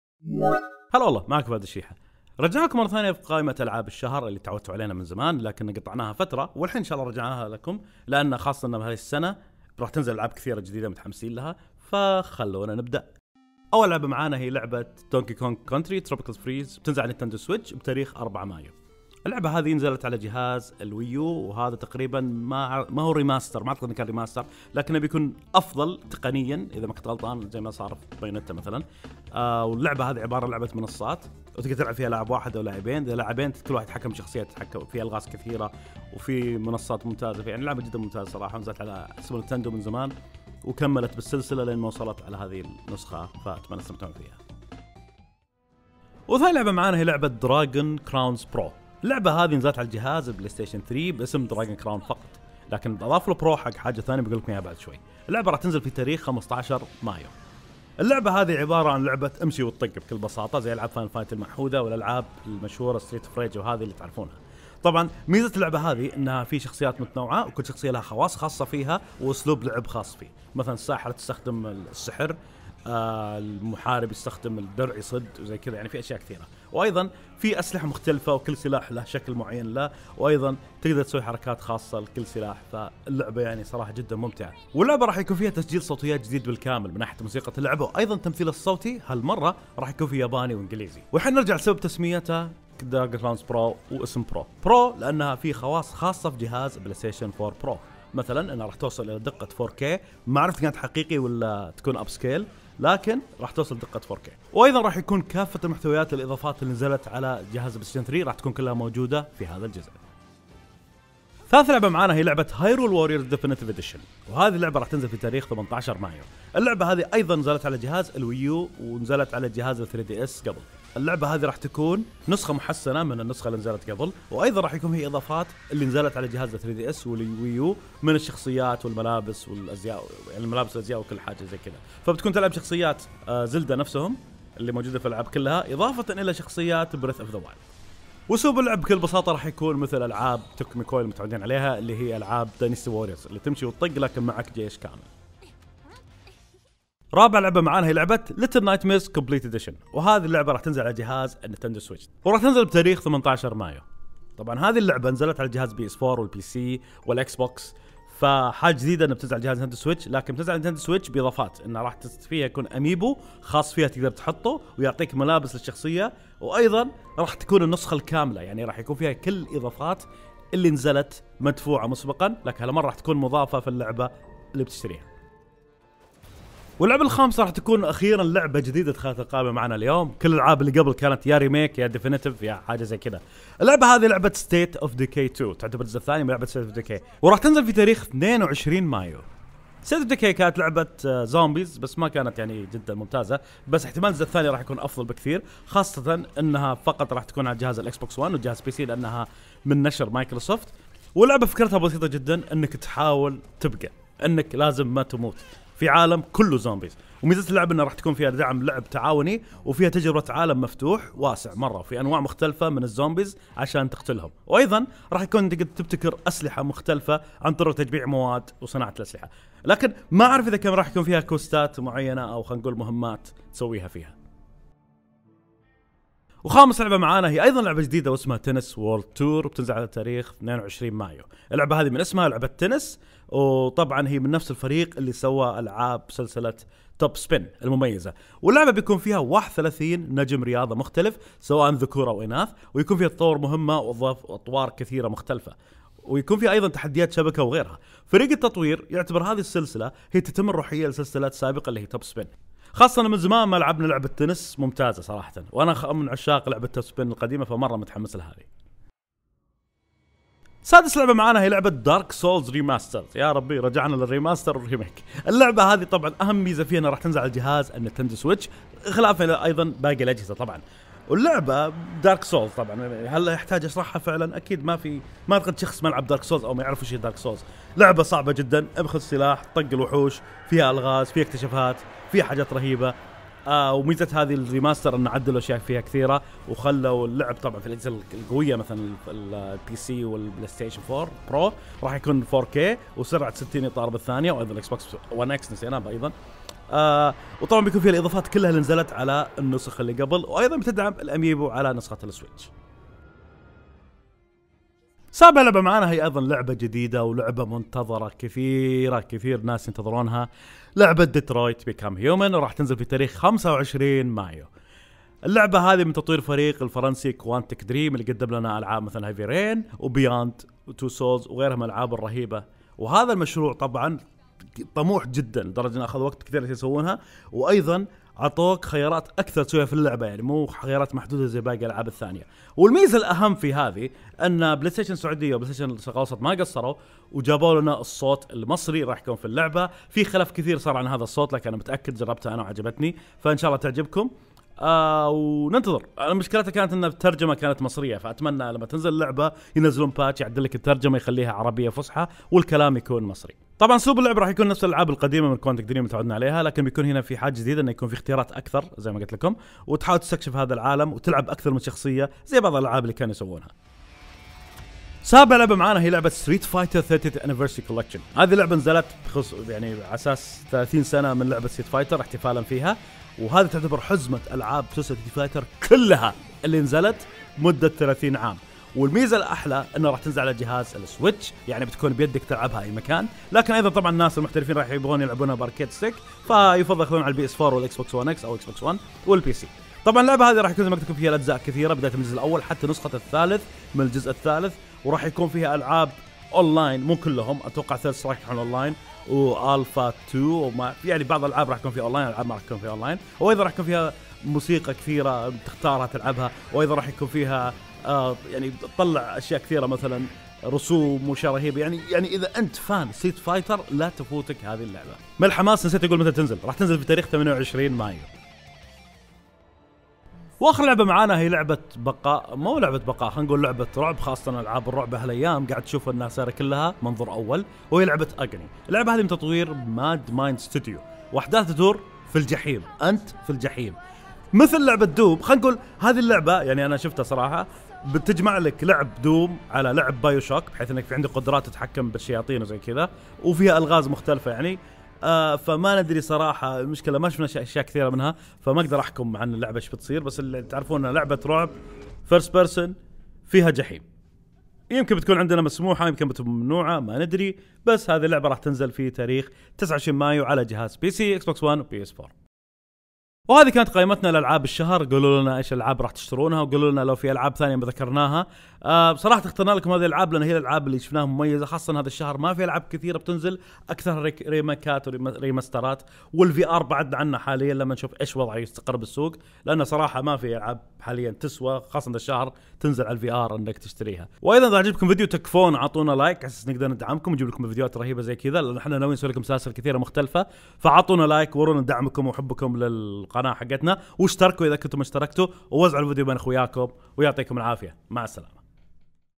ألعاب شهر مايو
في هذا الفديو نستعرض لكم العاب شهر مايو
أذا اعجبكم الفيديو وايضا اننا عدنا لسلسة العاب الشهر ادعمونا بالايك والاشتراك